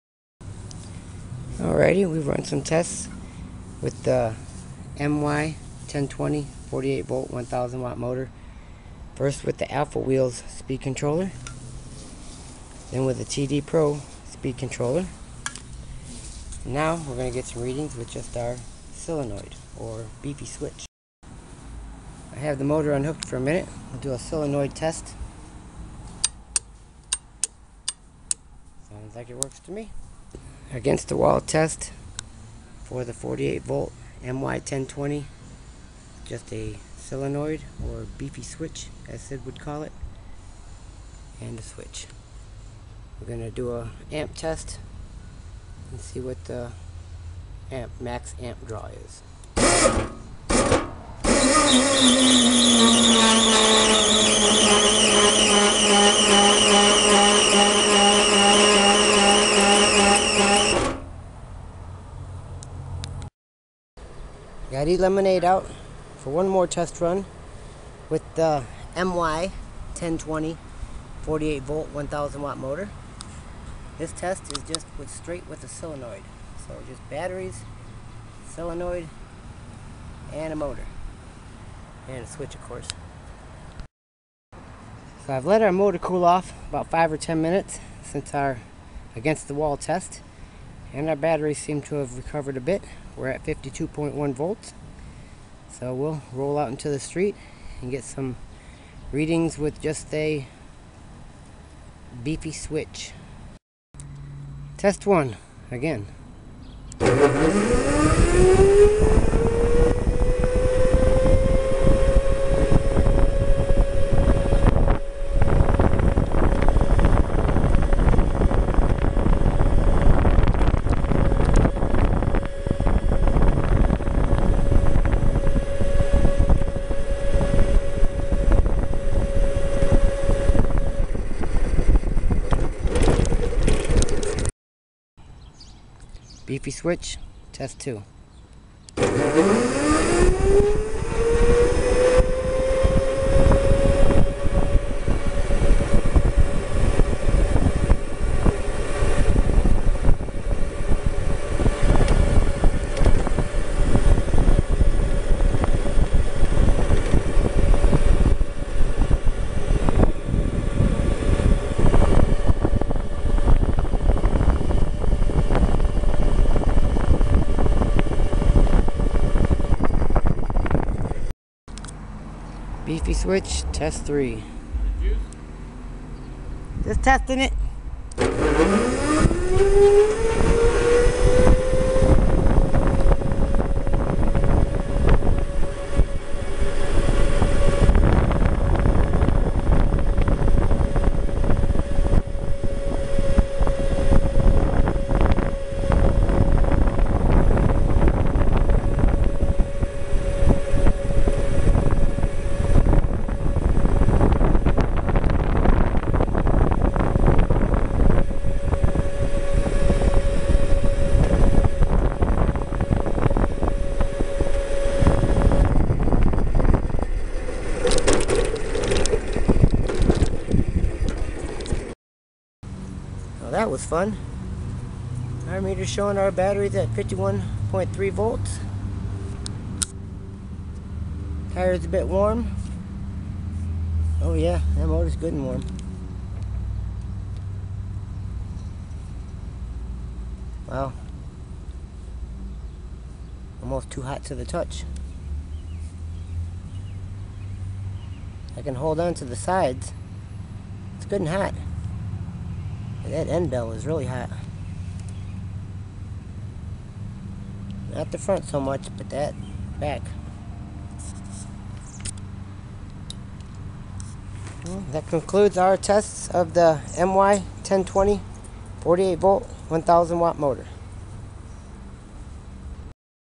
MY1020 48V 1,000W motor straight power no controller test
Third test of the MY1020 motor to gather some comparison data. This is a "Hold on to your Hat" test, the motor is wired direct (no speed controller) just the battery pack, a beefy switch & the motor.